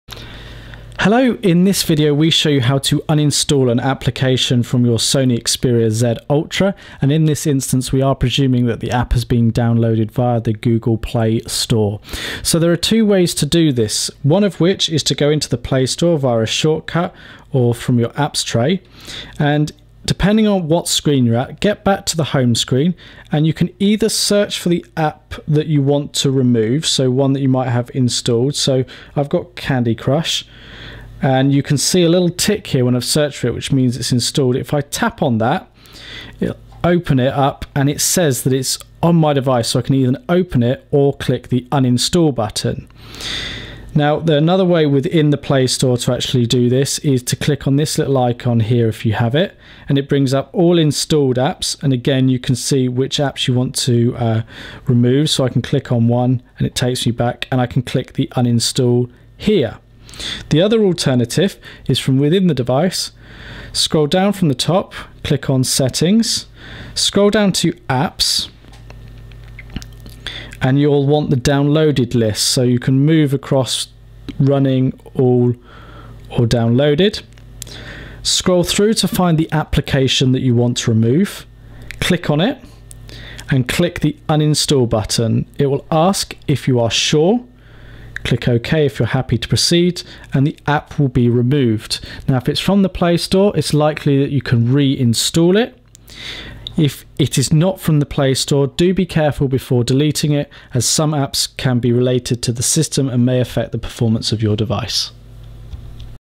How To Uninstall An Application - Sony Xperia Z Ultra
A video how to, tutorial, guide on uninstalling an app from your Sony Xperia Z Ultra.